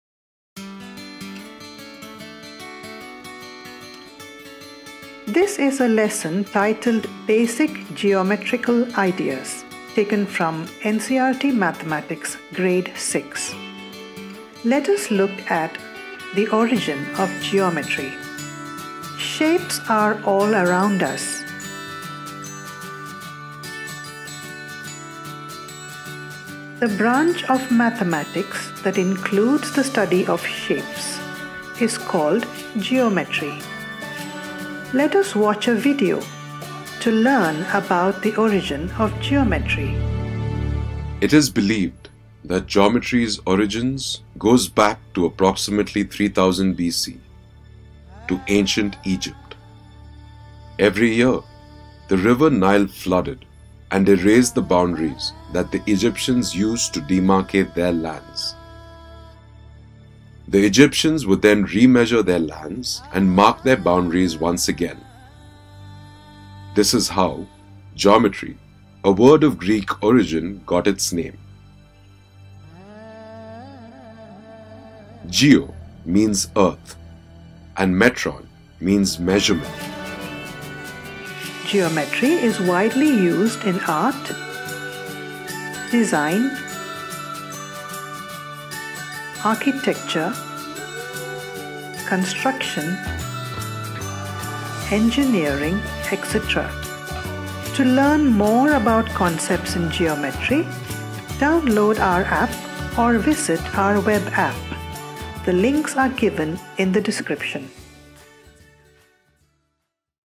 Origin of Geometry
It is believed that Geometry’s origin goes back to approximately 3000 BC, in ancient Egypt. Let us learn some interesting facts about 'Geometry'!